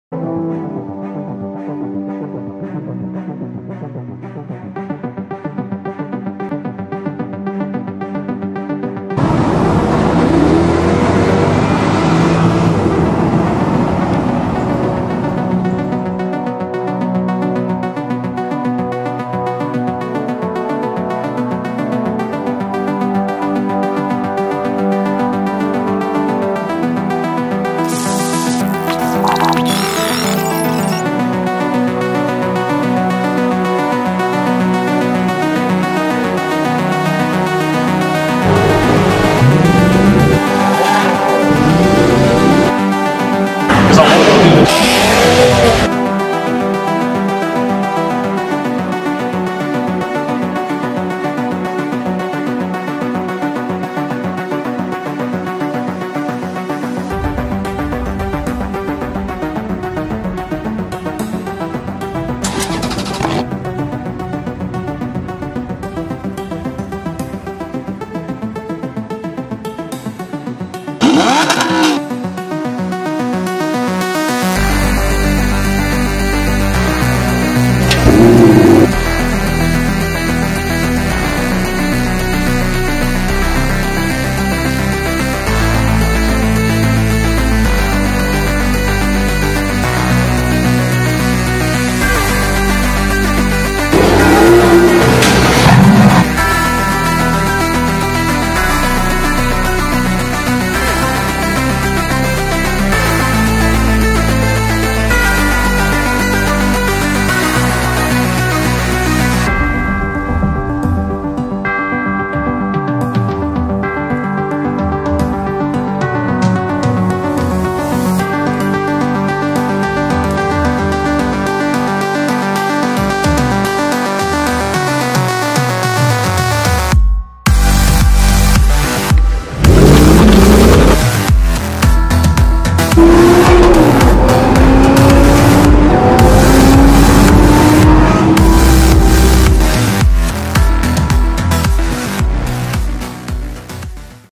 2015 in 2 minutes - TheCarGuy18's 2015
In this video you will see a 2 minute compilation of some of the stuff I got up to this year and of course there's some pretty awesome cars!